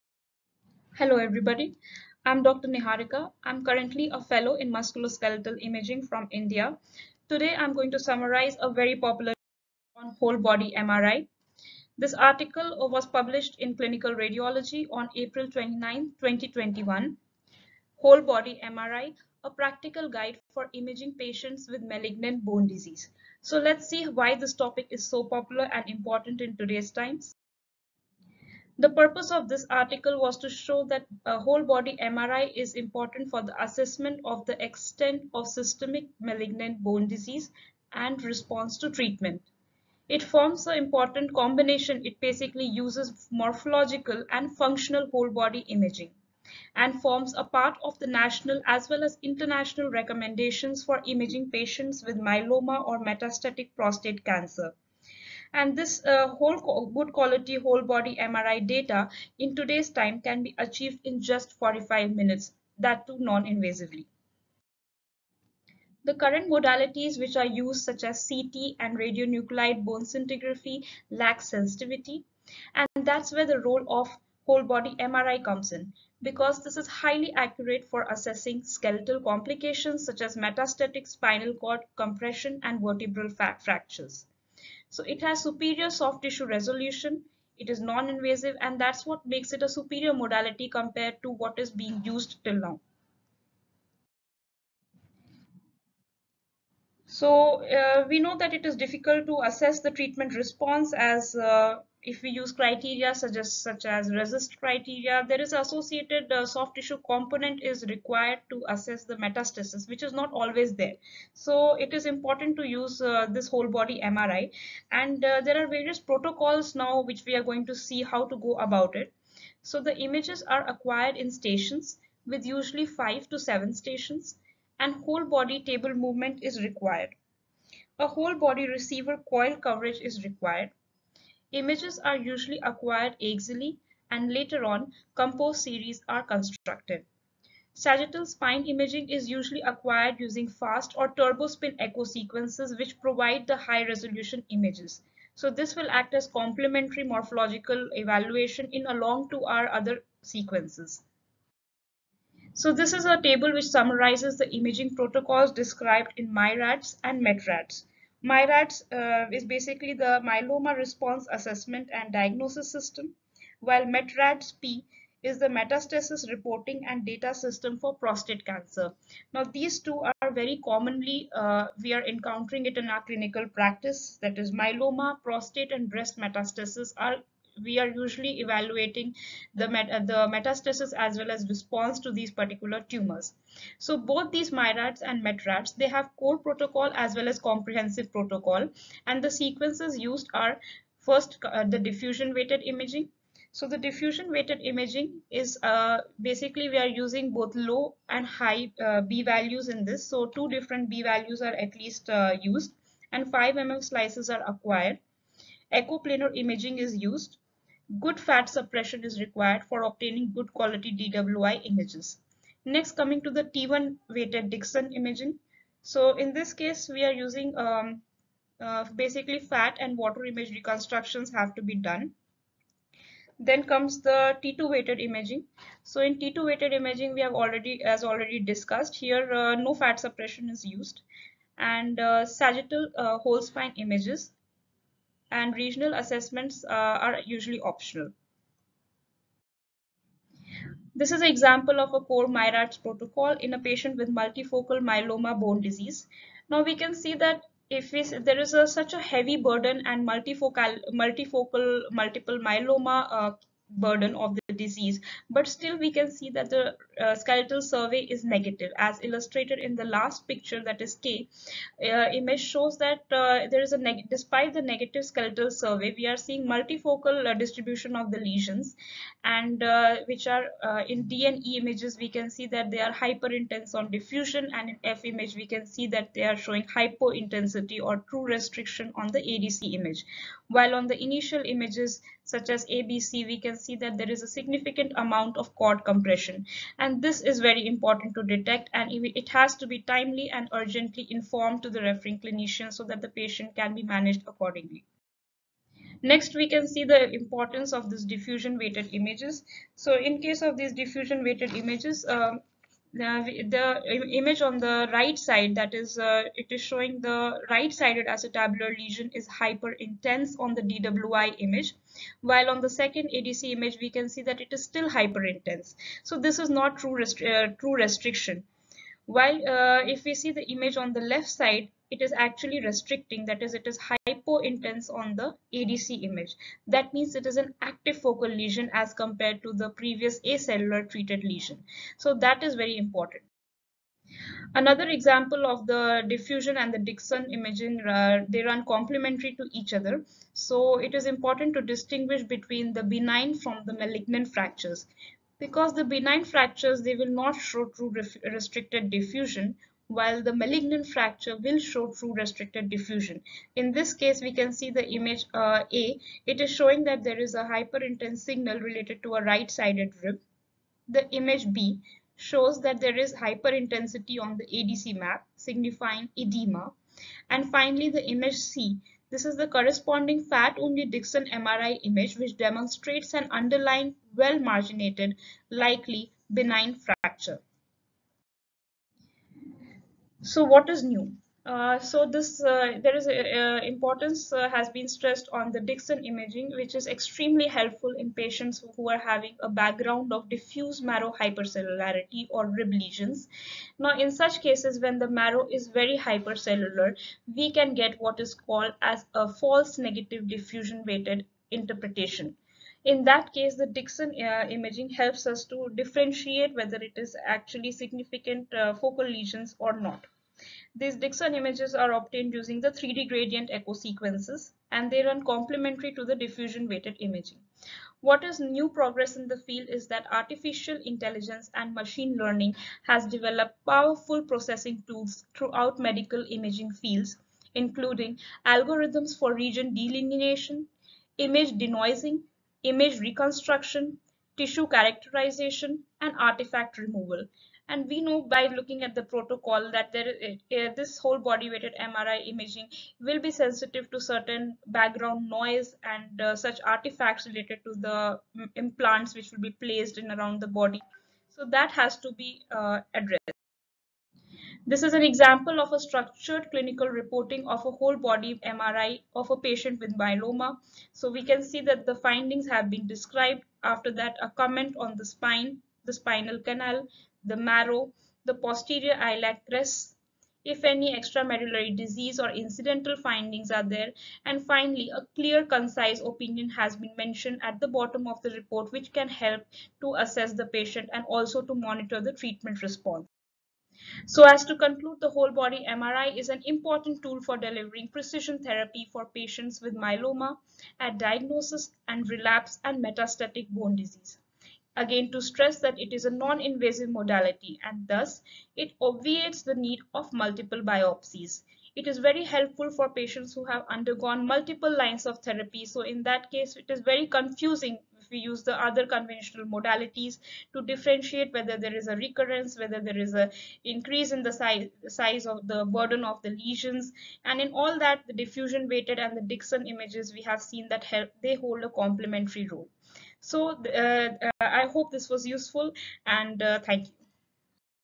Whole Body MRI: An update
Summary of a latest article on whole body MRI, Clinical Radiology; 2021